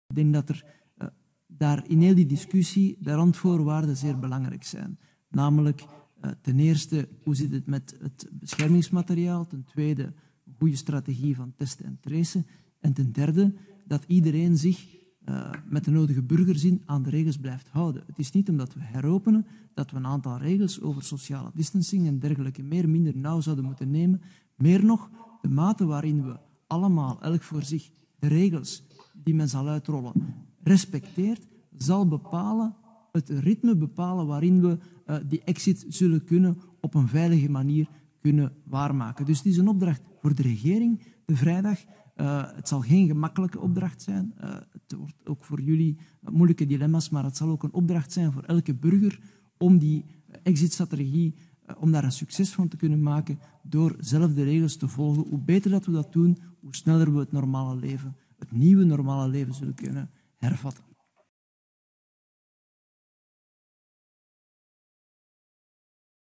Plenaire Vergadering 22 april - Exitstrategie Coronaciris
👉“De Nationale Veiligheidsraad zal vrijdag voor dilemma’s komen te staan om te bekijken hoe we de ‘heropening’ kunnen verzoenen met de veiligheid van onze burgers.
In heel deze discussies zijn de randvoorwaarden erg belangrijk: voldoende beschermingsmateriaal , goede strategie van testen en traceren en de nodige burgerzin.
Want de mate waarin we allemaal, elk voor zich, de regels die men zal uitrollen respecteert, zal het ritme bepalen waarin we die exit op een veilige manier kunnen waarmaken.”